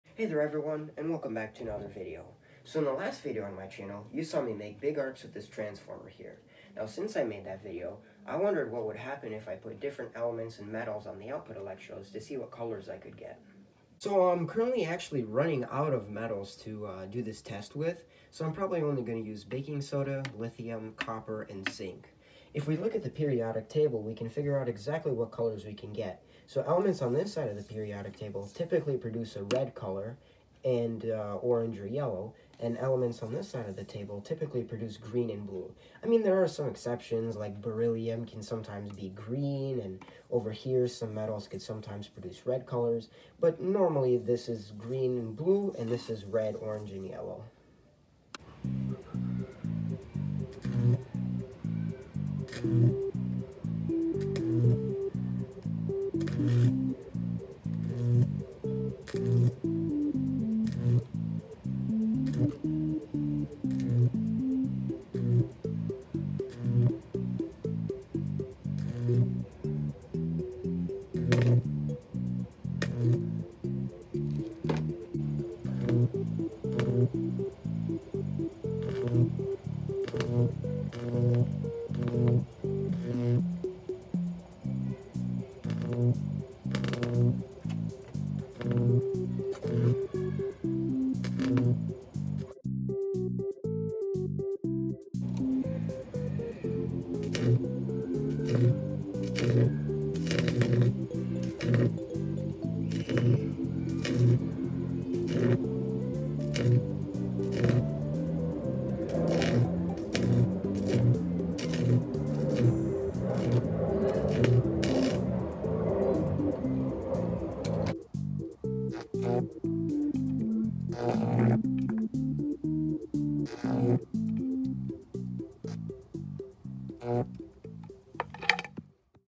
Changing The Color Of ELECTRICITY!
I use my chemistry skills to change the color of High
Voltage Arcs.
The Colors I got where Red from Lithium, Yellow from Sodium
(Baking Soda), Green from Copper and Bluish Purple from Zinc.

By the way some of the colors where pretty hard to see on camera,
Probably because the original orange color of the Arc was washing
Out the colors that where being produced by the metals, and also I
Wanted to test what color Lead and Americium would produce in the
Arc, but I didn’t want toxic and radioactive Gas to be Produced, so
Maybe when I get better safety equipment I can test that.

I would also like to point out that the sodium ions from the baking soda
Made the arc size the largest, while the zinc ions made the arcs the smallest.